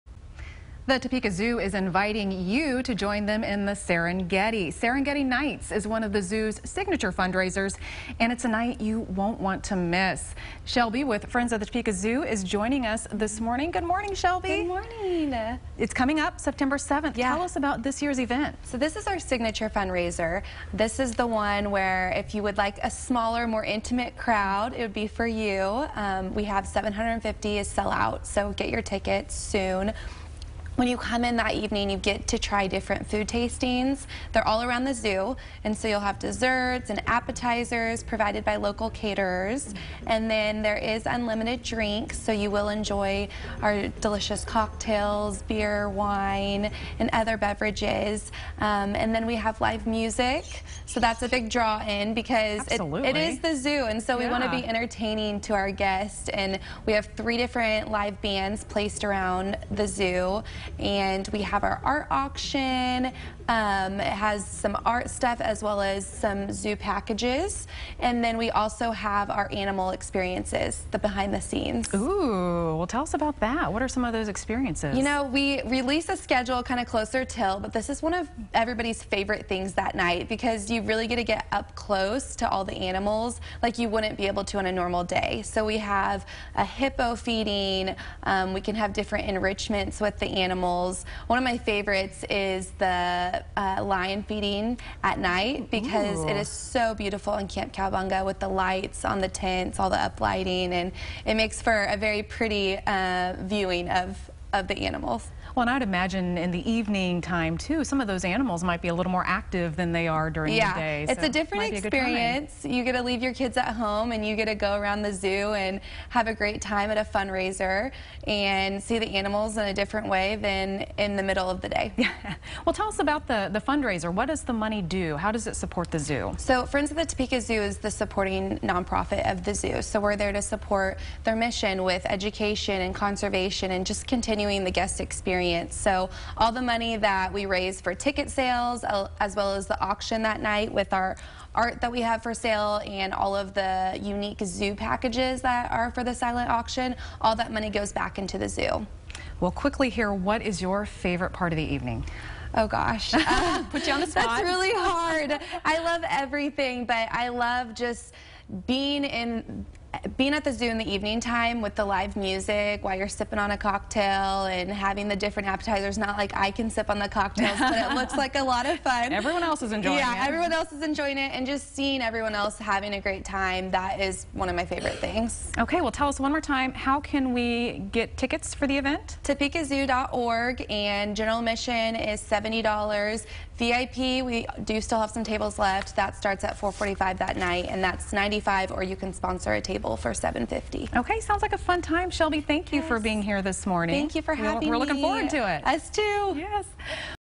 The Topeka Zoo invites you to Serengeti Nights on Sept. 7
Shelby Revelle with Friends of the Topeka Zoo sits down with FOX 43's Erin La Row to talk about the zoo's signature fundraiser, Serengeti Nights on September 7.